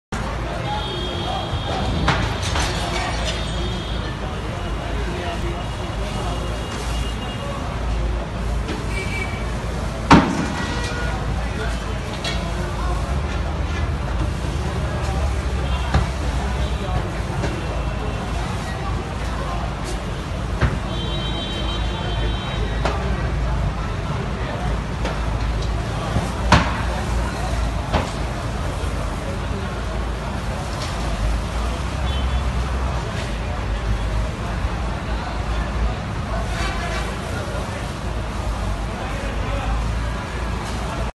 Tabaahi, Huge Fire At Prince department store Multan Cantt
My channel is ff teev fun and entertainment Huge Fire in Prince Departmental Store, Almost Sara Samaan Jal Gya, Koiy Jani Nuksan Nahi Hoa